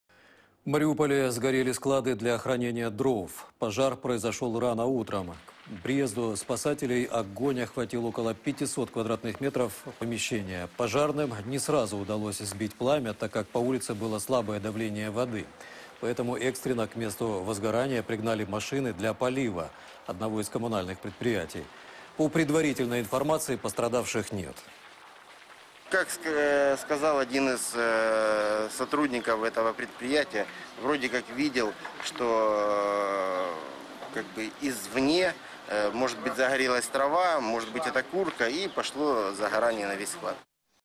ТК Донбасс - В Мариуполе сгорели склады для хранения дров.
Пожар произошел рано утром. К приезду спасателей огонь охватил около 500-т квадратных метров помещения. Пожарным не сразу удалось сбить пламя, так как по улице было слабое давление воды. Поэтому экстренно к месту возгарания пригнали машины для полива  одного из коммунальных предприятий. По предварительной информации пострадавших нет.